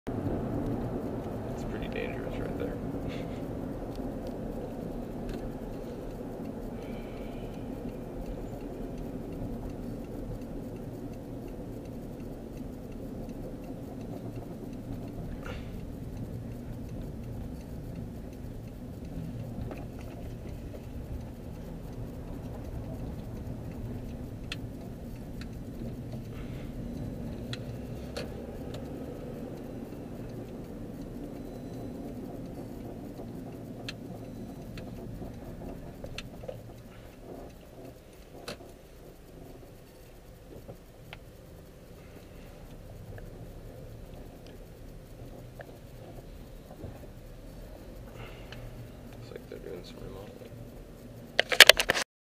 2012.09.28 Daily Driving: Job, Bank, Carwash, Food [8/10]
Going to/from my current job. Then went to the bank, got a carwash, ate some sandwich, then drove home. Driving around now with MORE commentary! Sometimes even special Guests. Just an example video from my Contour HD camera to show off the video quality to friends. You might want to turn down your speakers, the microphone is lacking.